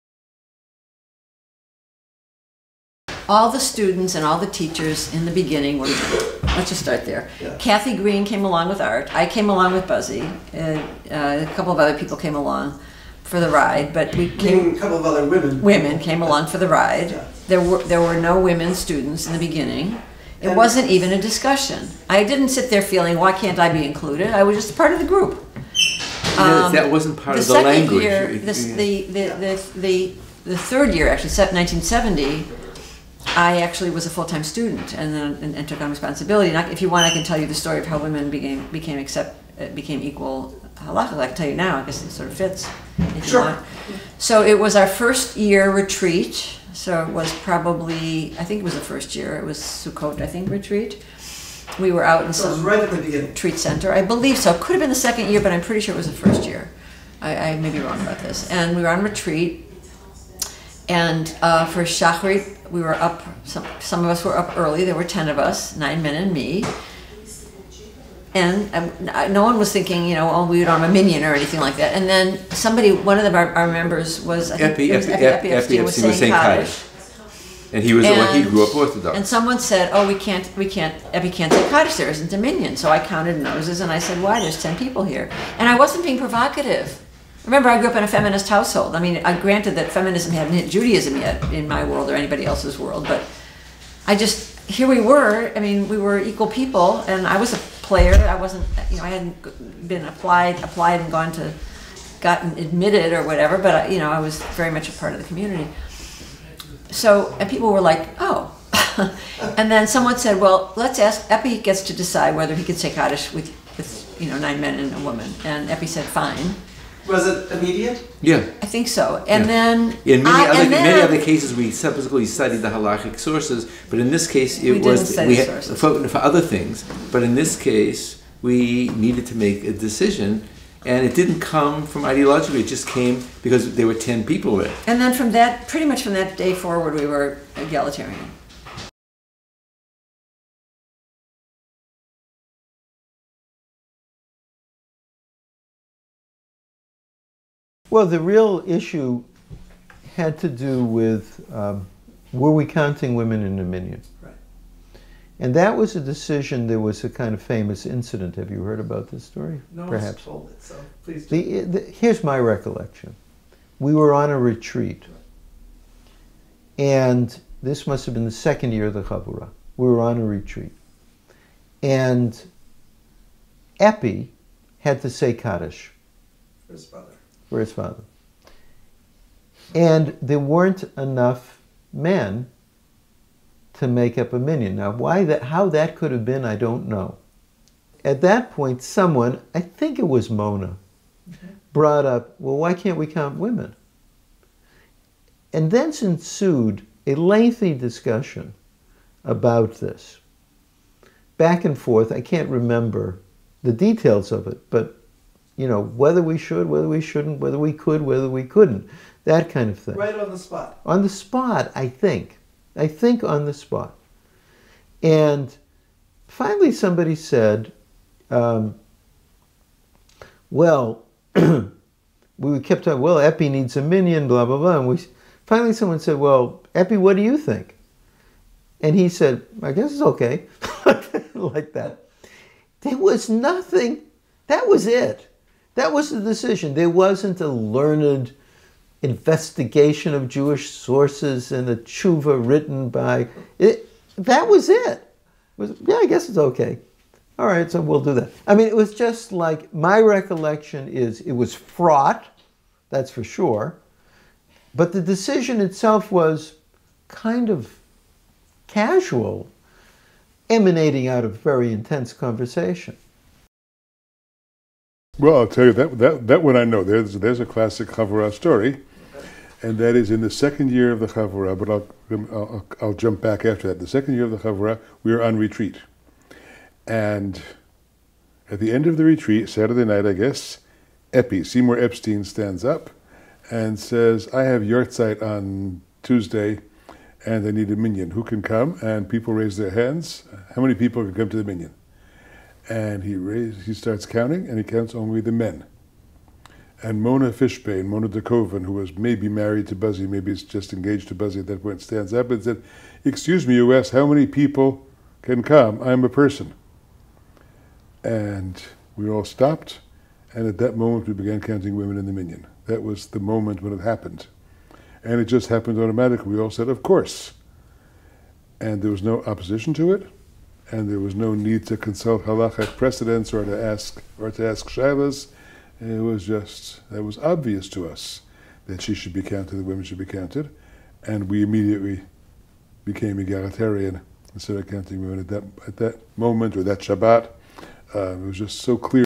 One Change, Three Stories: Women in the Minyan at Havurat Shalom
Short series of clips from three Jewish Counter Culture Oral History interviews used as part of a conference presentation by the filmmaker, Jayne Guberman. This video is part of the Jewish Counter Culture Oral History Project.